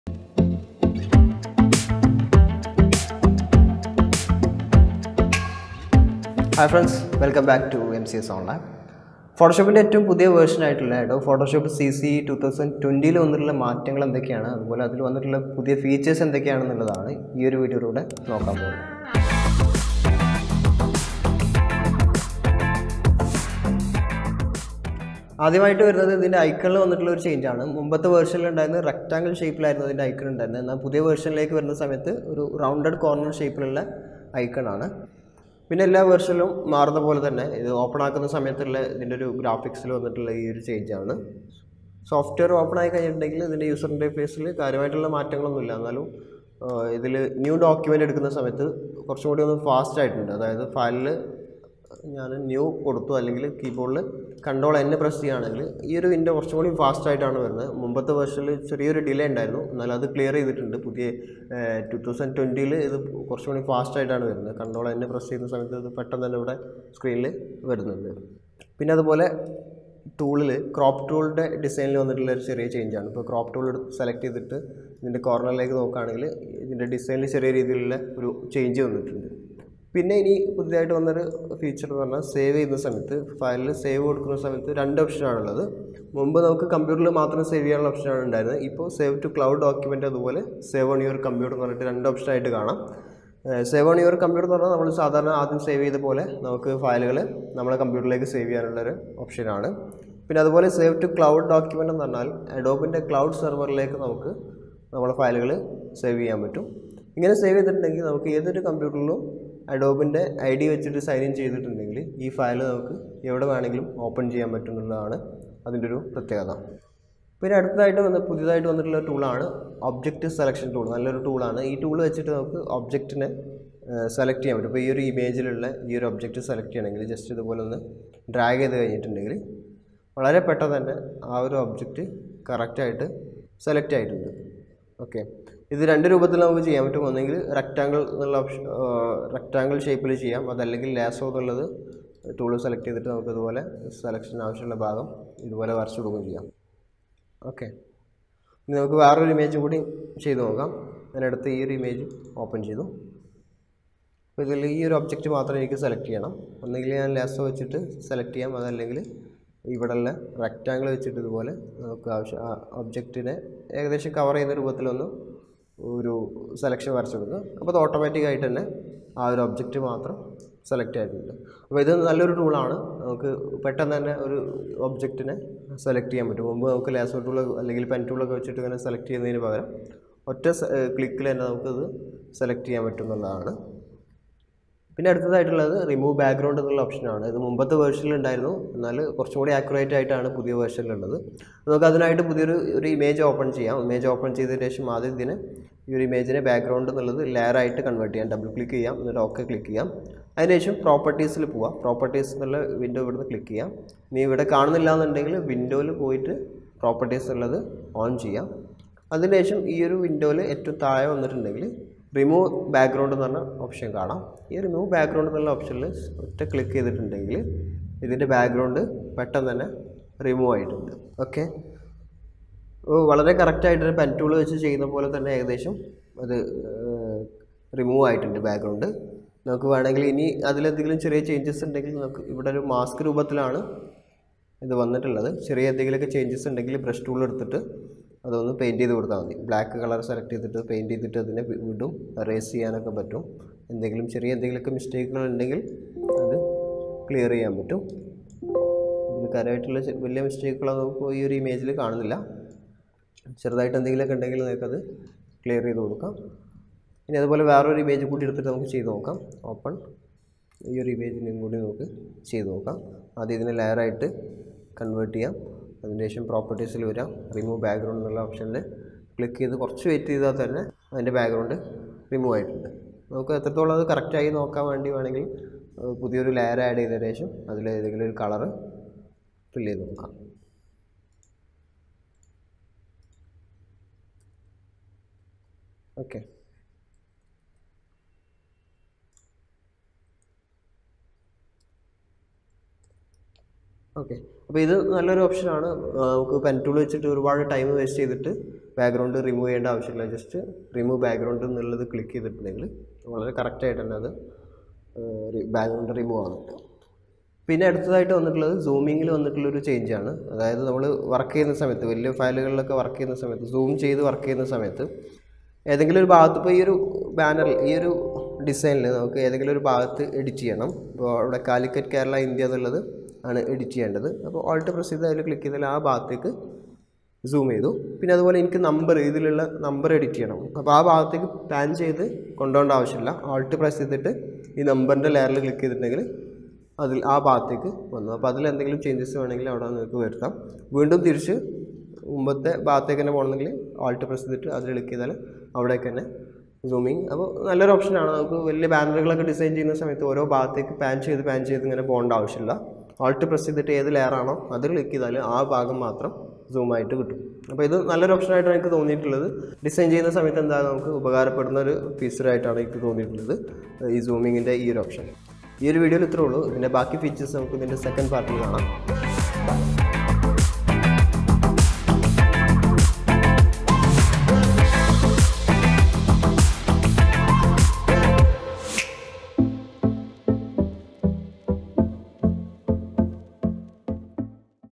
Photoshop CC 2020 New features in Malayalam Part 1 | Graphic Designing Malayalam
Photoshop CC 2020 New features in Malayalam
Fast new document |Fancy crop tool |Save to cloud | Object selection tool |Remove background | Zoom to layer content

mcs onlive by salih mc